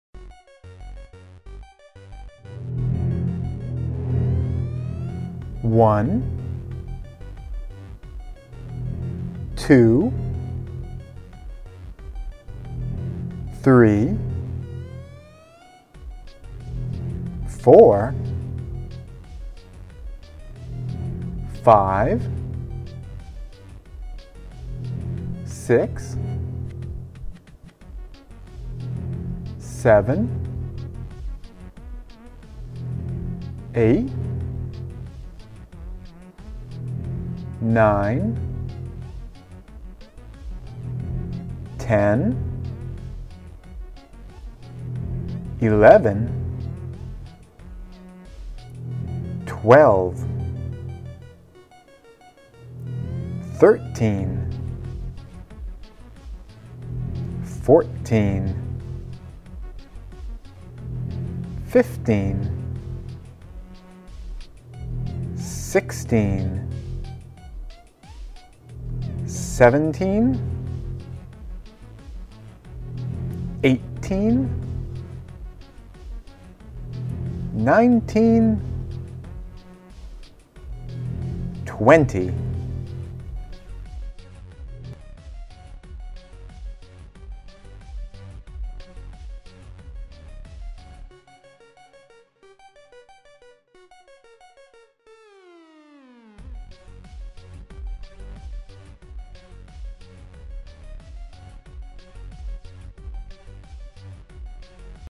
Monster Pacman Teaching to Count 1 to 20 in English Language and Numbers Video for Kids
Watch monster pacman while you learn. You've found the best educational cartoon video!  Learn about alphabets, math, counting, numbers, colors, music, and more with these educational cartoon parade for kids!